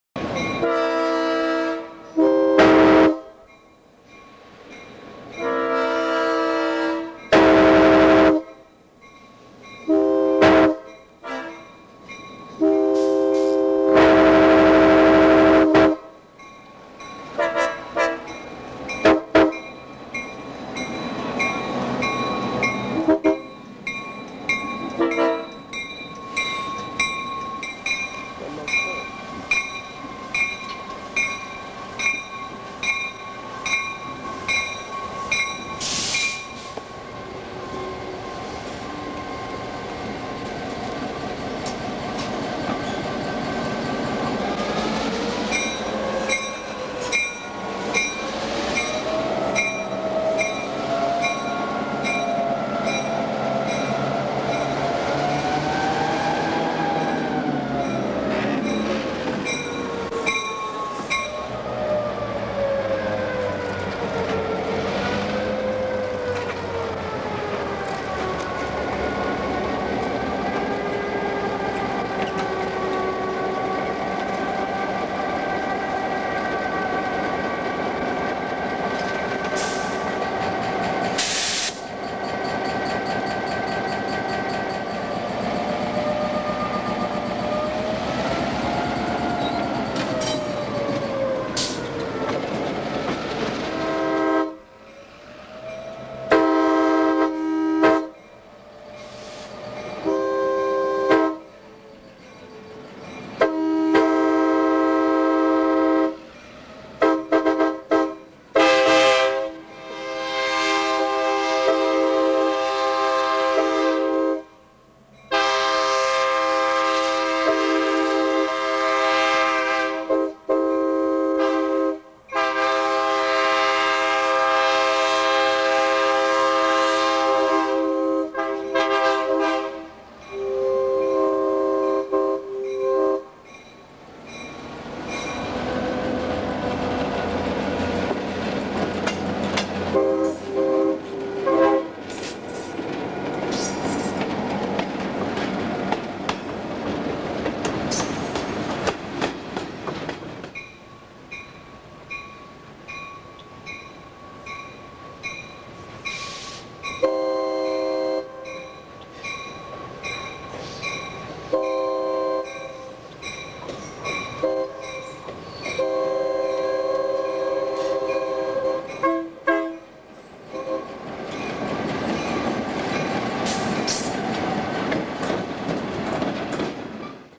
Finger Lakes Railway in Auburn, NY - Summer 1999
During the earlier years on the FGLK, it wasn't uncommon to see some interesting power.  Buffalo & Pittsburgh high nose GP9 #203 is seen heading west through Auburn with a S5T horn and mainstay low nose GP9  #1703 switches the steel plant and rounds up a few cars to head west to Solvay, NY.